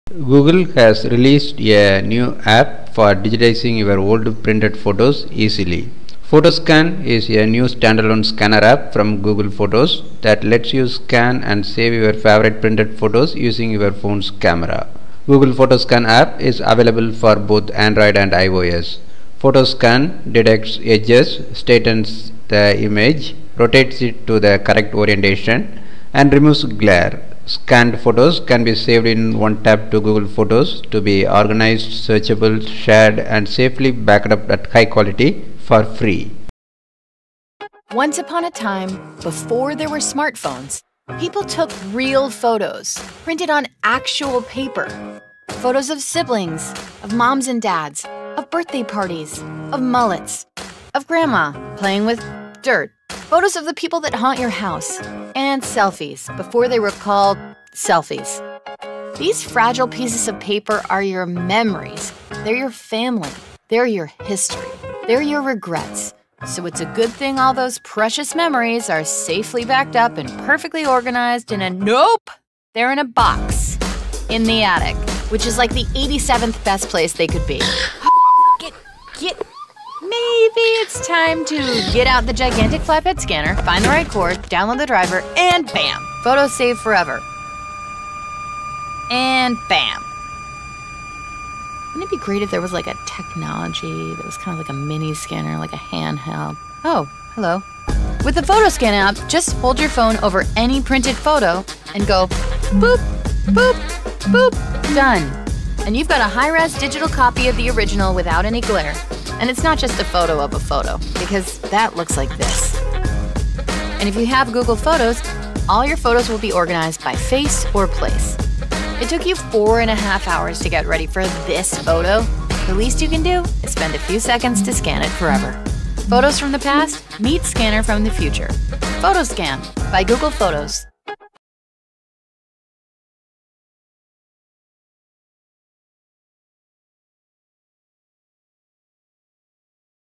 Google's PhotoScan app can help you to scan your old photos easily without glare | QPT
Google has released a new app for digitizing your old printed photos easily.

PhotoScan is a new standalone scanner app from Google Photos that lets you scan and save your favorite printed photos using your phone’s camera.

Google PhotoScan app is available for both  Android and iOS.

PhotoScan  detects edges, straightens the image, rotates it to the correct orientation, and removes glare. Scanned photos can be saved in one tap to Google Photos to be organized, searchable, shared, and safely backed up at high quality—for free.

Video Source/Courtesy: Google